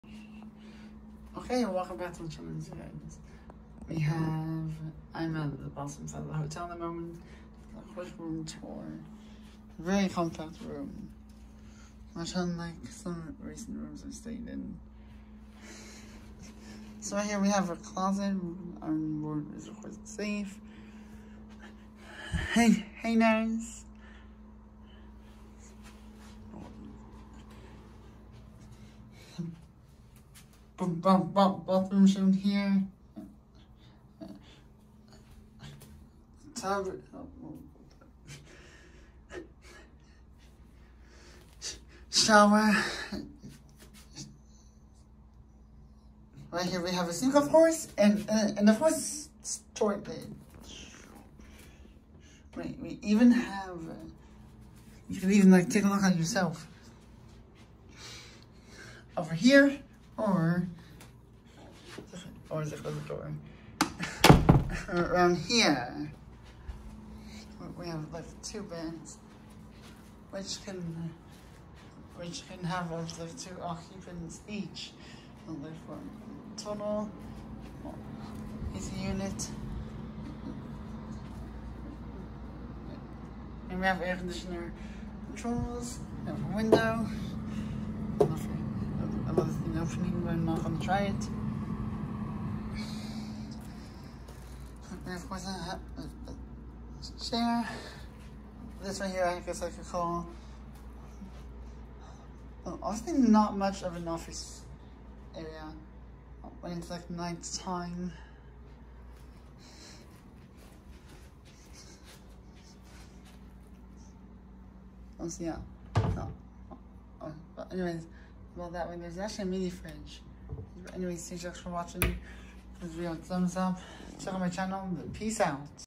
(60fps) Hotel Room Tour - Boston Park Plaza
This is one of the SMALLEST hotel rooms I've EVER stayed in! Enjoy the video.